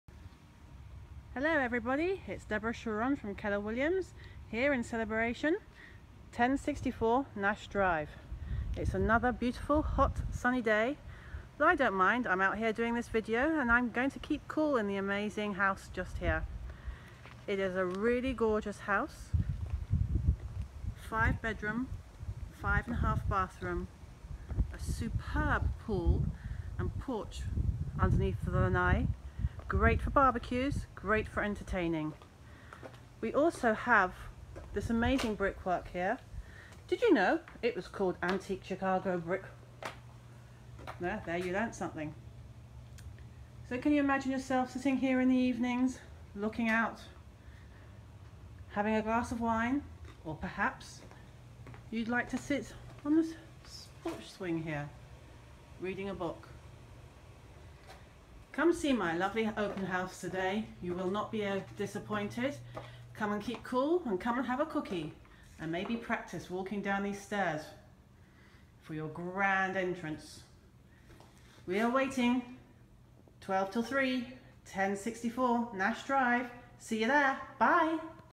Open House Celebration
Nash Drive Celebration
5 bedroom 5/1 bath 3966sqft
Do you not to miss this beautiful home come see it today 07/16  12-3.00pm